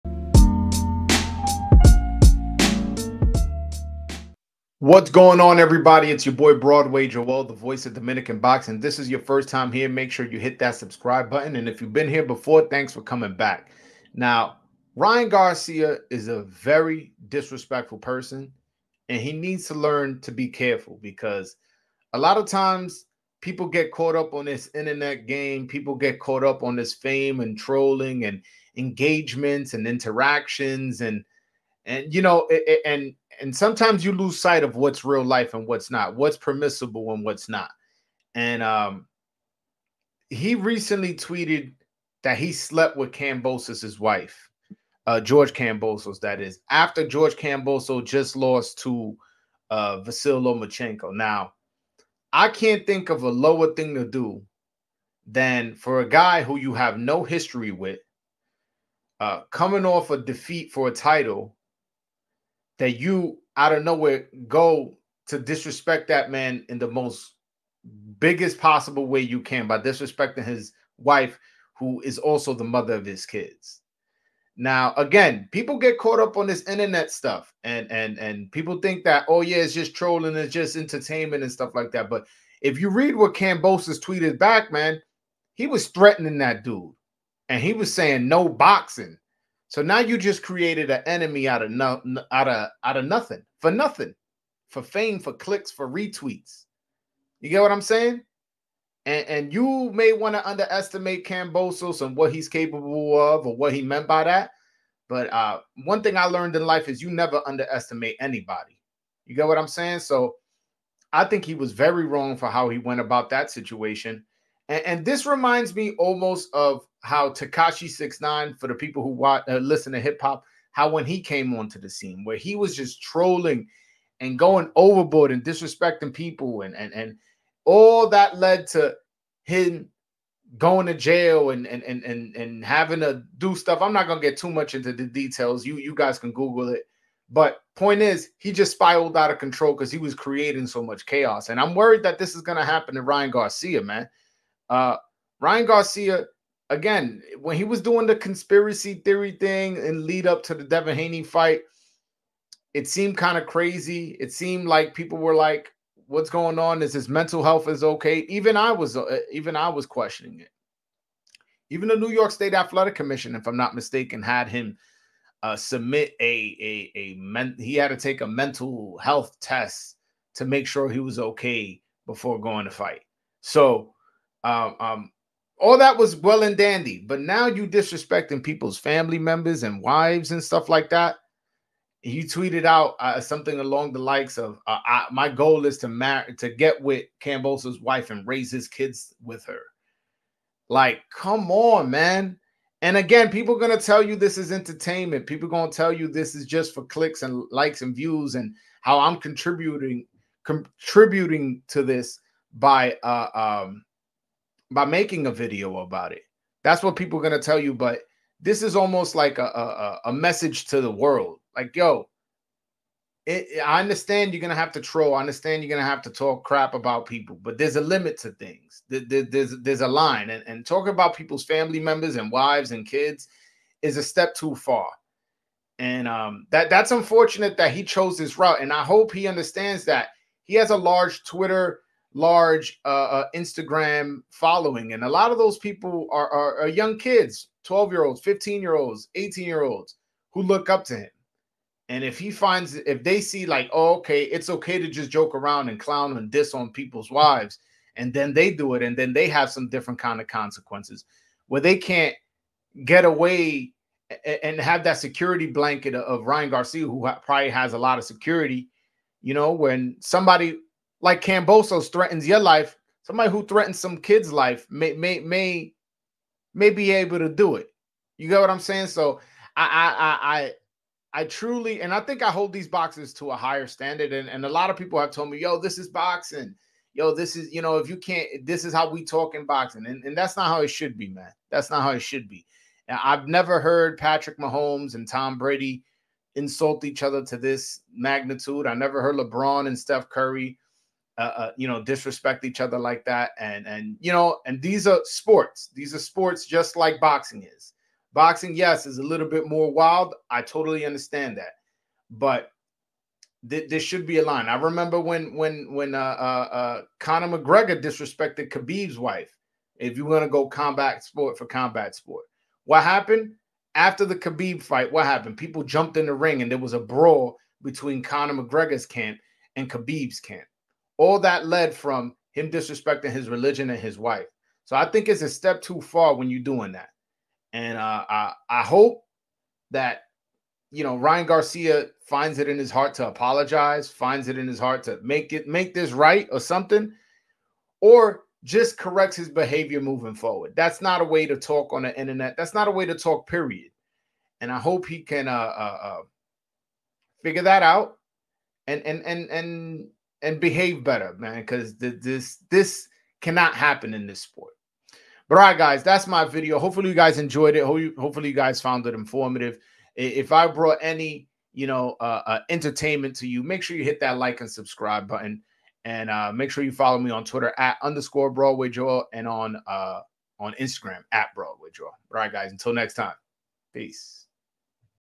Ryan Garcia is The Tekashi 69 of Boxing
Ryan Garcia is one of boxing's most polarizing figures. Sometimes too polarizing. In this video we talk about Garcia's disrepect towards George Kambosos' wife and his similarities to rapper Tekashi 69.